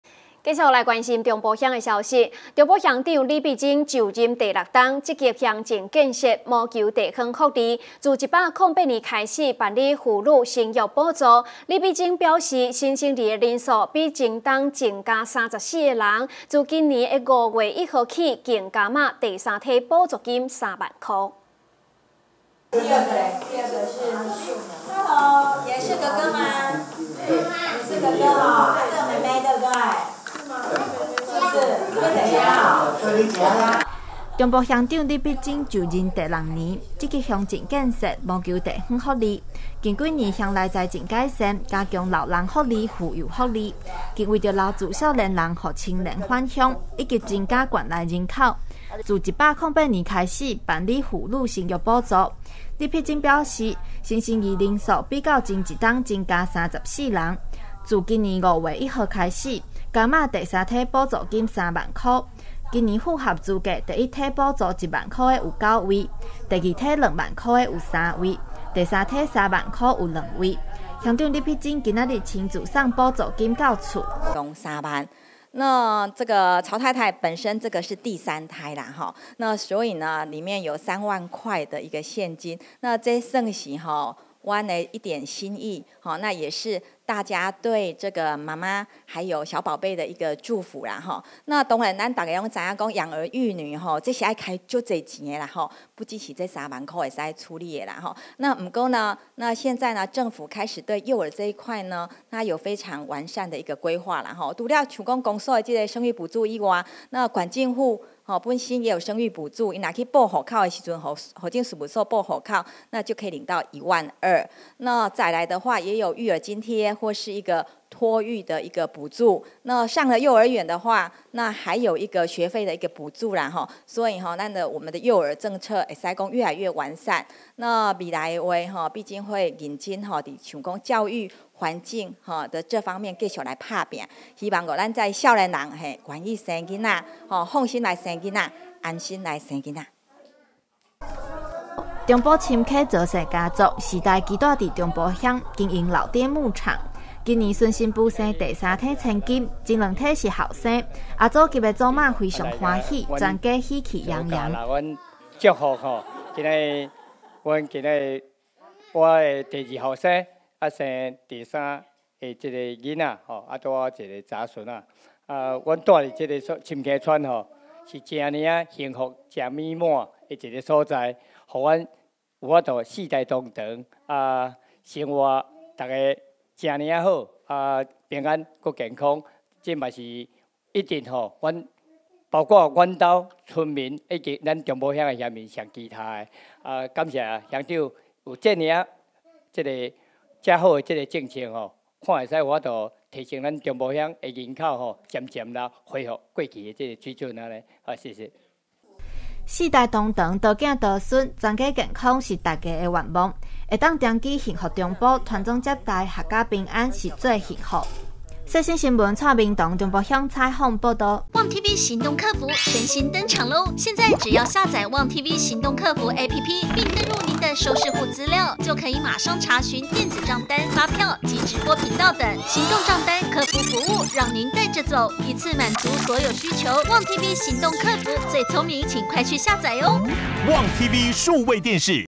世新新聞 鼓勵生育 中埔鄉婦女生育補助到第三胎
109年6月09日 (二) 世新新聞

鼓勵生育 中埔鄉婦女生育補助到第三胎

中埔鄉長李碧菁就任第六年，積極鄉政建設，謀求地方福利，近幾年鄉內財政改善，加強老人福利、婦幼福利，更為留住年輕人讓青年返鄉，以及增加鄉內人口，自108年開始辦理婦女生育補助，李碧菁表示，新生兒人數較前年增加34人，自今年五月一日起，加碼第三胎補助金三萬元，今年符合資格第一胎補助1萬有9位，第二胎2萬有3位，第三胎3萬有2位，鄉長李碧菁今天親送補助金到家，中埔深坑曹姓家族，四代同堂多子多孫，接到幸福紅包深感長居幸福中埔。